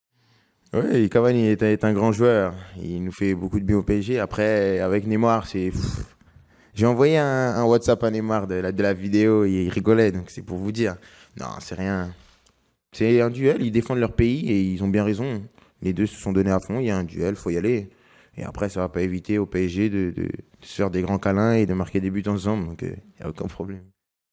Mbappé: "No hay problemas entre Cavani y Neymar"
Edinson Cavani y Neymar, compañeros en el PSG se enfrentaron en la fecha de amistosos FIFA con sus selecciones. Kyllian Mbappé habló de la relación de sus compañeros en el club parisino.